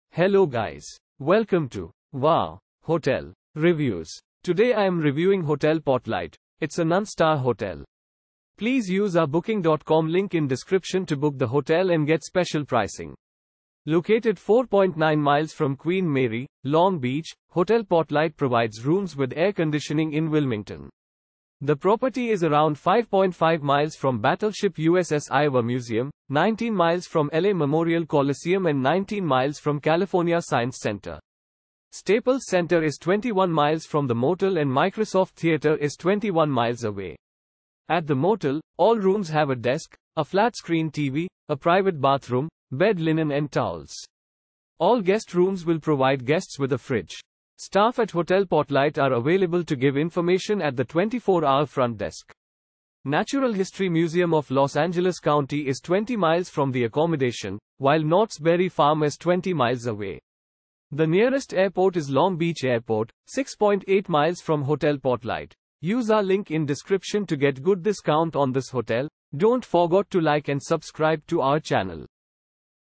Hotel Portlight Review - Wilmington , United States of America
Hotel Portlight

Located 4.9 miles from Queen Mary, Long Beach, Hotel Portlight provides rooms with air conditioning in Wilmington. The property is around 5.5 miles from Battleship USS Iowa Museum, 19 miles from LA Memorial Coliseum and 19 miles from California Science Center. Staples Center is 21 miles from the motel and Microsoft Theater is 21 miles away.
At the motel, all rooms have a desk, a flat-screen TV, a private bathroom, bed linen and towels. All guest rooms will provide guests with a fridge.
Staff at Hotel Portlight are available to give information at the 24-hour front desk.
Natural History Museum of Los Angeles County is 20 miles from the accommodation, while Knotts Berry Farm is 20 miles away. The nearest airport is Long Beach Airport, 6.8 miles from Hotel Portlight.